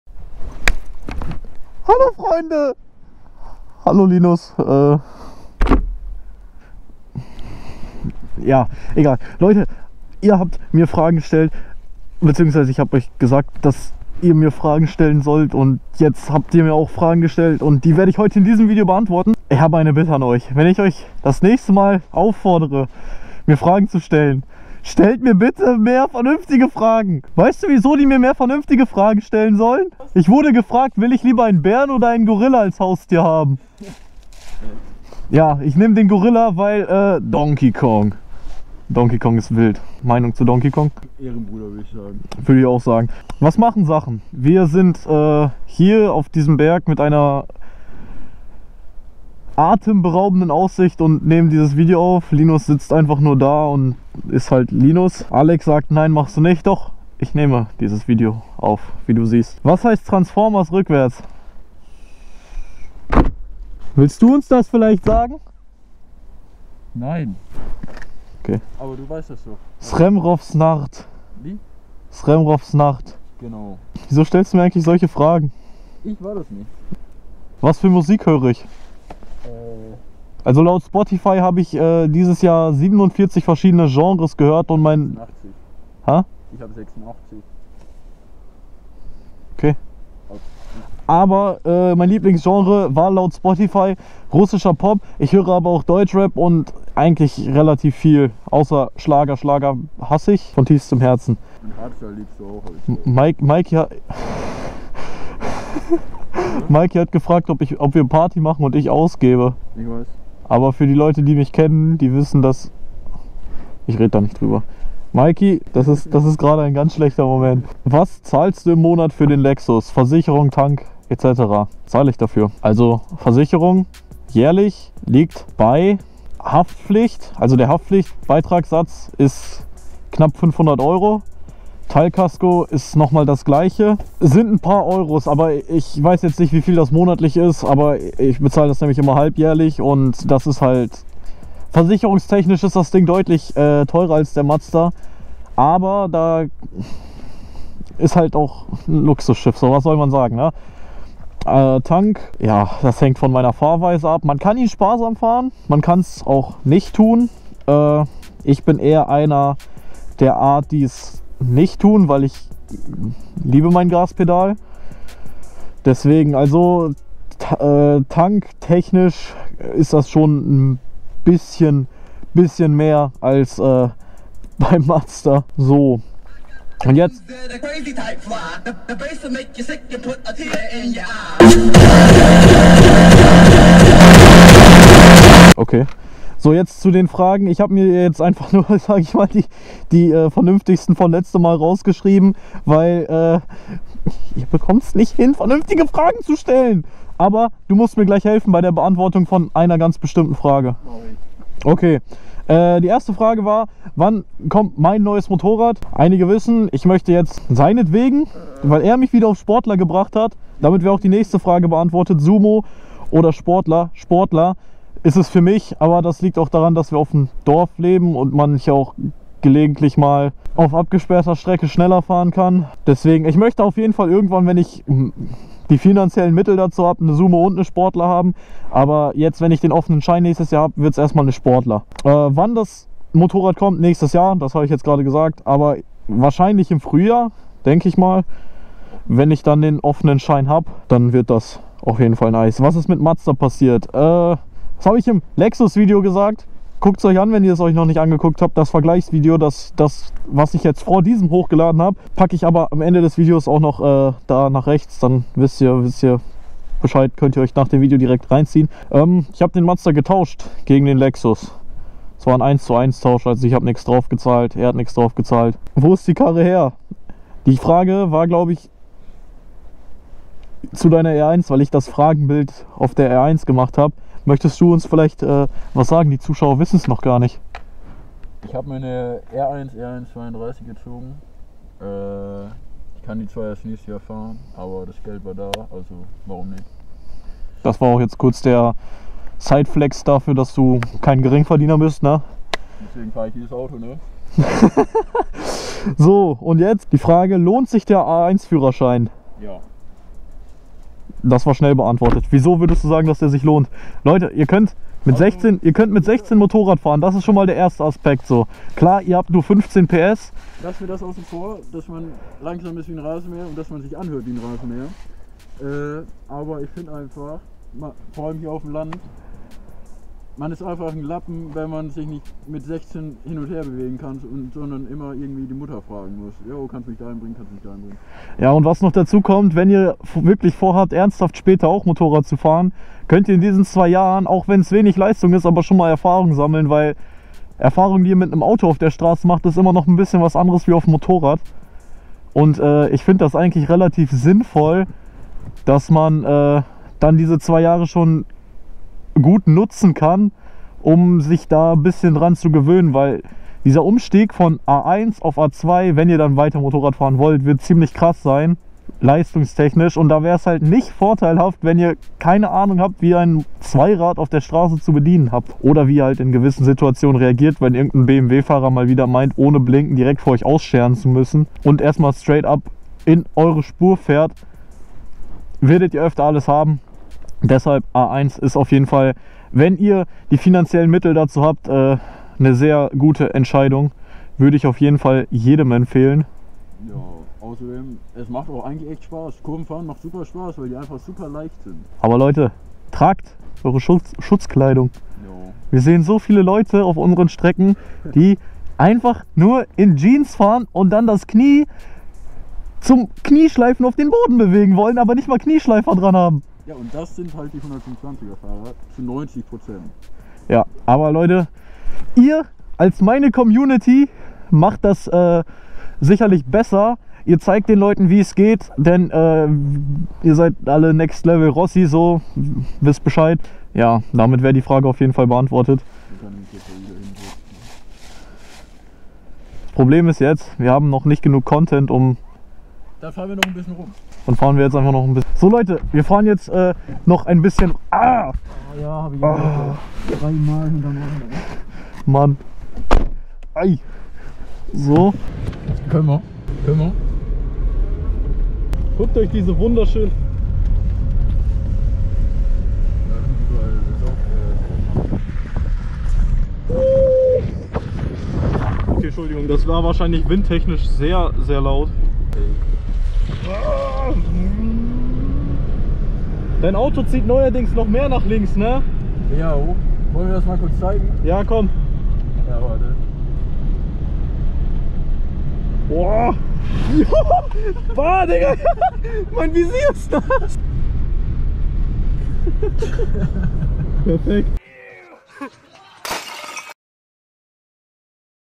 Lohnt sich der A1-Führerschein? | Eure (unnötigen) Fragen #2 | FAQ
Heute mal wieder eure meistens unnötigen Fragen beantworten.
Macht einfach einen Motorradführerschein das lohnt sich in jedem Fall.

Motorrad: KTM LC4 690 (A2)
Kamera: GoPro Hero 7 Black
Helm: Scorpion Exo 510 Air

bereitgestellt.